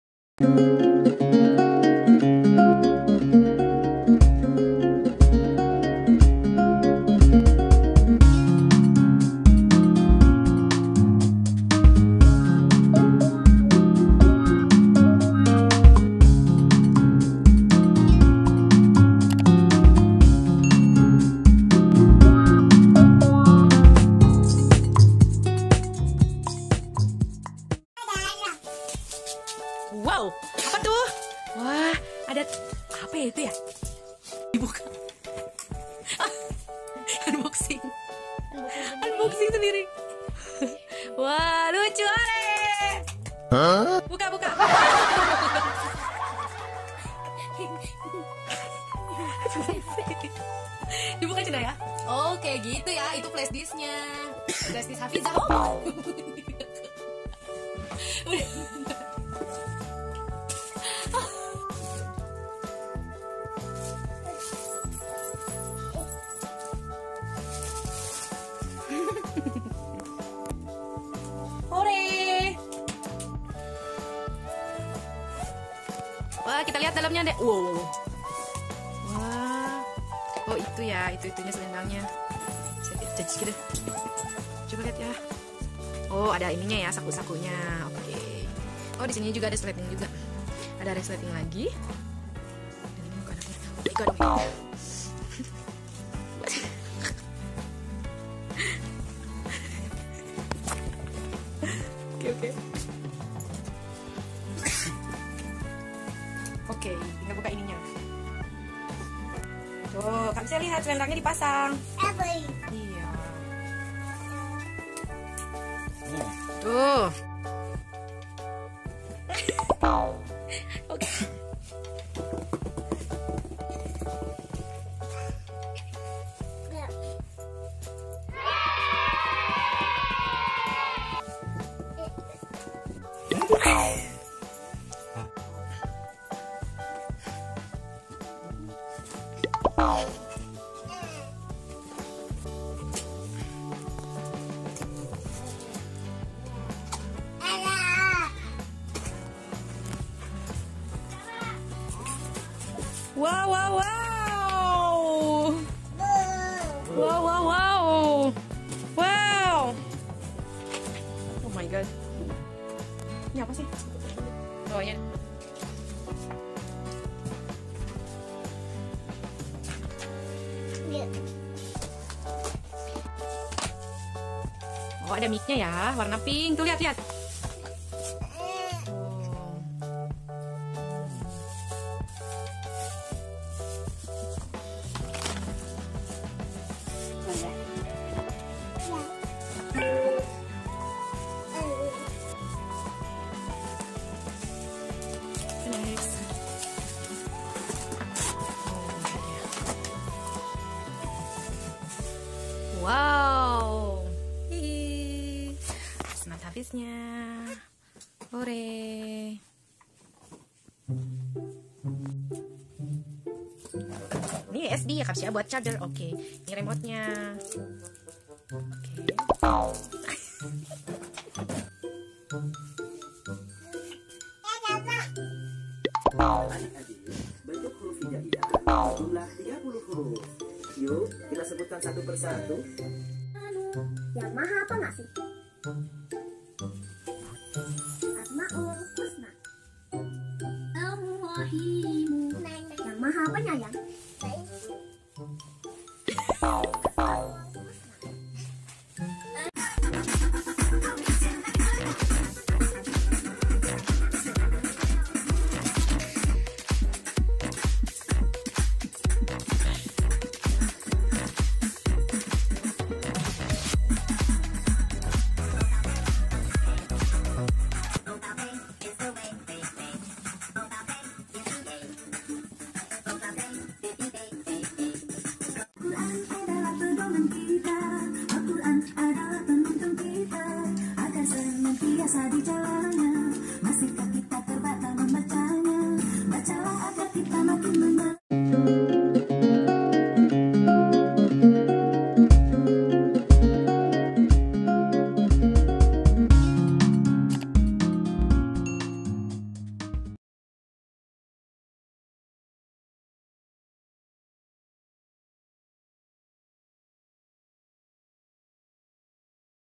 UNBOXING SMART HAFIZ | TAS + FLASHDISK HAFIZAH | Versi terbaru|touchscreen|rekomendasi buat anak
Kali ini kinema buka semua isi paket nya hingga  coba tasnya dan buka flashdisk hafizahnya yang lucu gemes banget ampe mau dimakan😂

Sebenernya smart hafiz ini diperuntukan untuk usia 2 tahun keatas, memang kami beli kecepetan tapi gapapa itung itung invest selagi dipakai sambil diawasi dan waktunya diatur biar ga kelamaan juga😍

link pembelian disitu yakkk☝

link medsosku. 😆

(video ini diambil saat usia kinema 10 bulan jalan 11, baru bisa diupload karena emak ini baru sempet editnya hehe)

Semoga bermanfaat dan sedikit menghibur 🤩